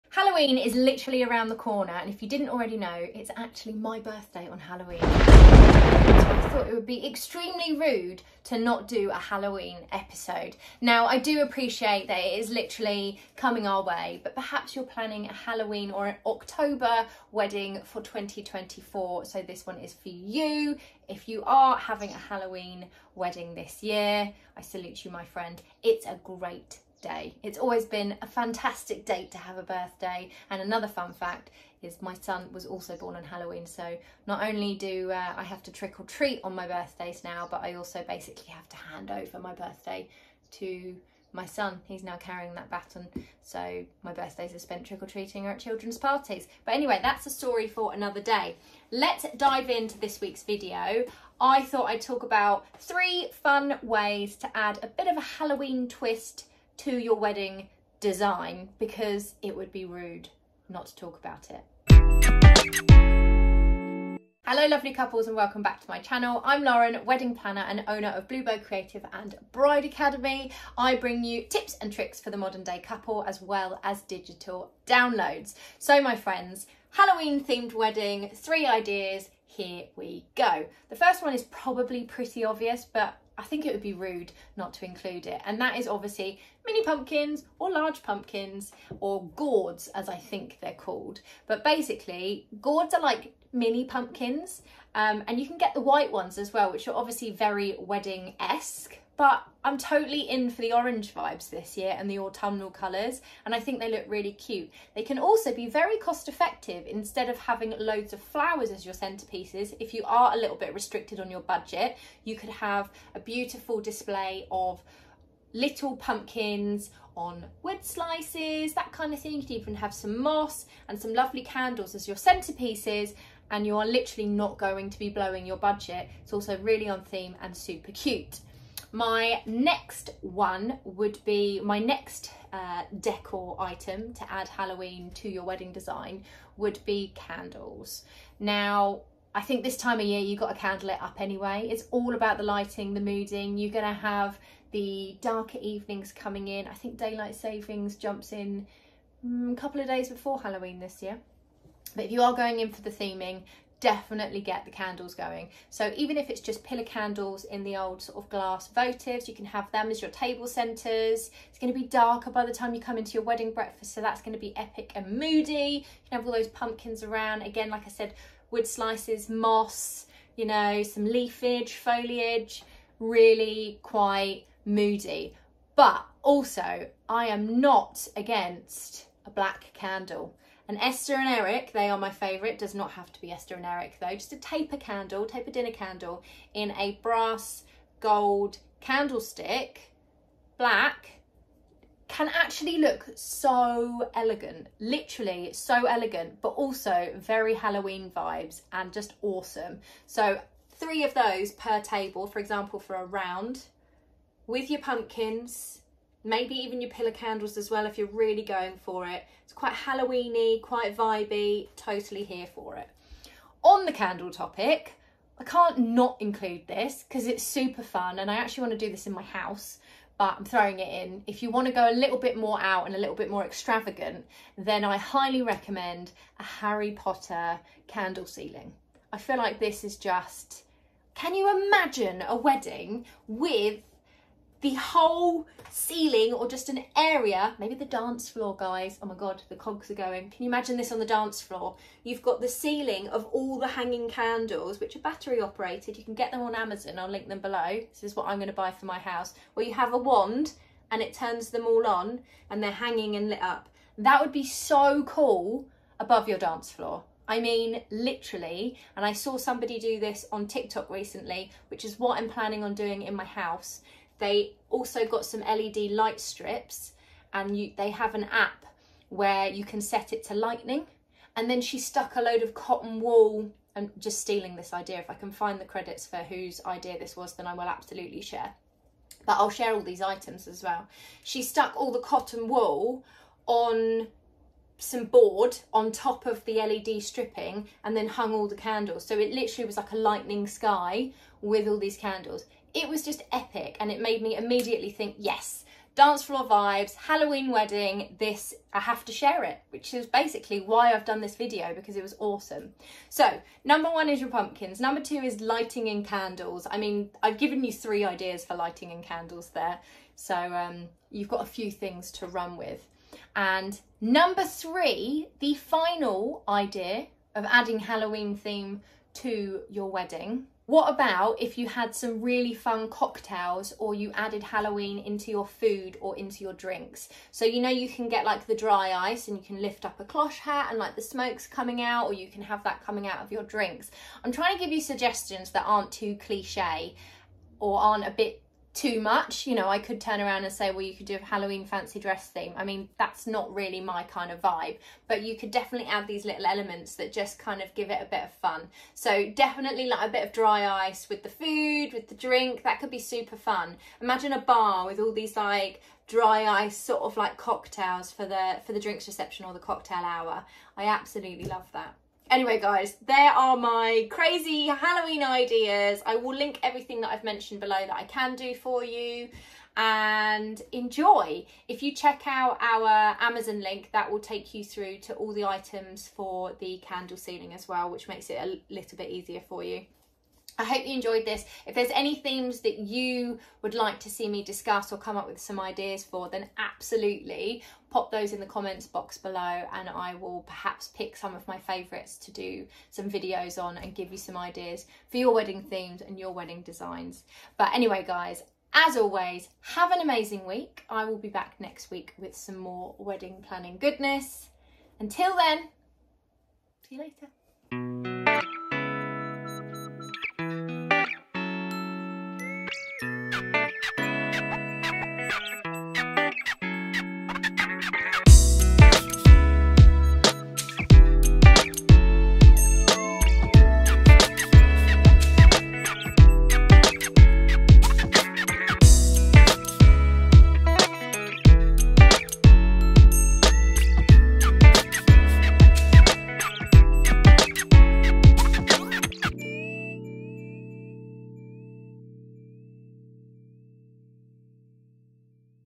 3 Halloween Themed Wedding Ideas
3 Halloween Themed Wedding Ideas

Let's dive into some theme ideas. With Halloween just around the corner, I thought it was a great theme to start with. It's also my birthday on Halloween so even better :)

3 Halloween decor ideas that don't look tacky and add fun and interest to your wedding design

all links below for you

Original tiktok credit to @bradley_chandler